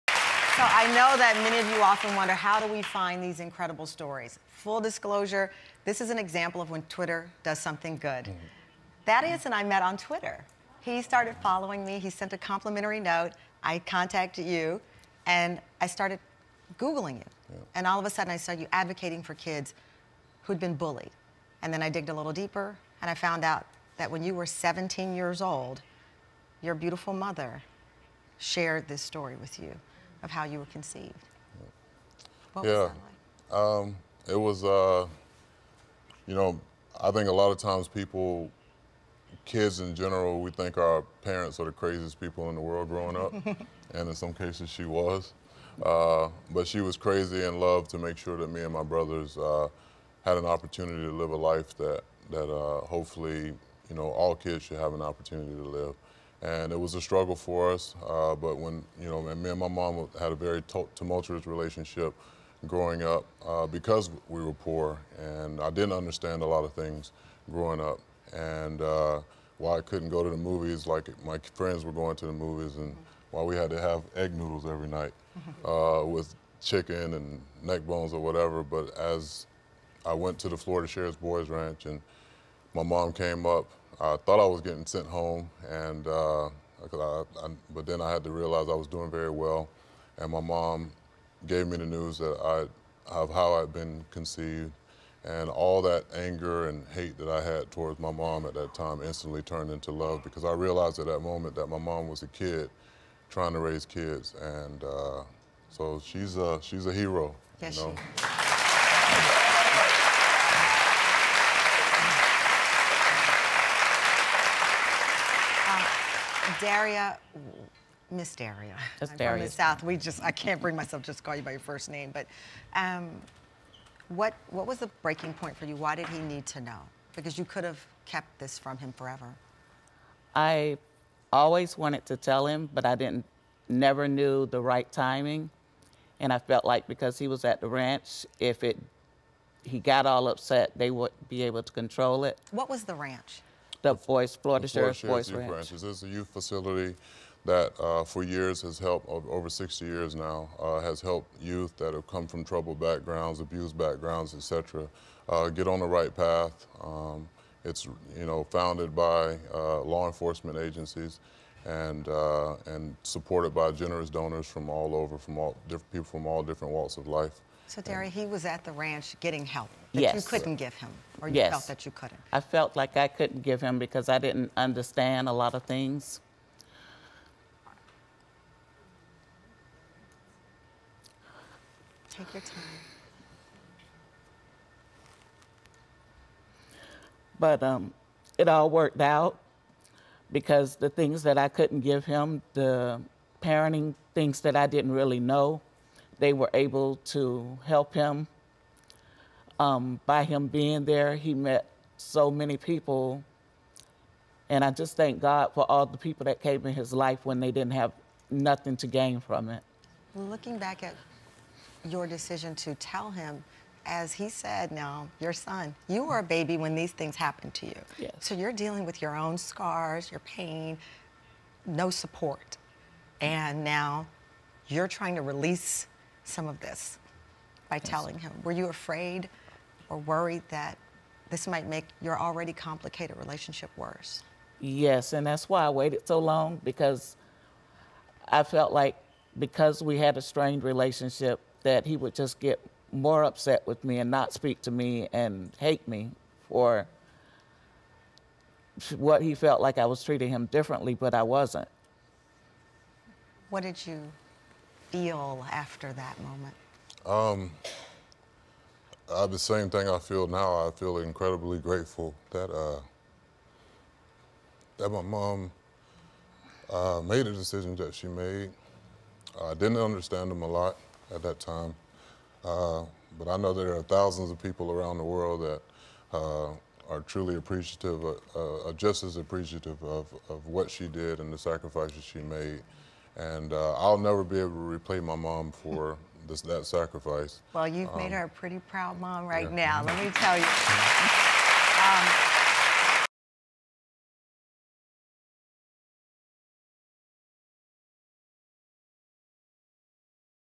From Stereotype to Prototype: Thaddeus “Titus O’Neil” Bullard
WWE superstar Thaddeus “Titus O’Neil” Bullard and his mother, Daria detail the pain of their pasts which led them to triumph today.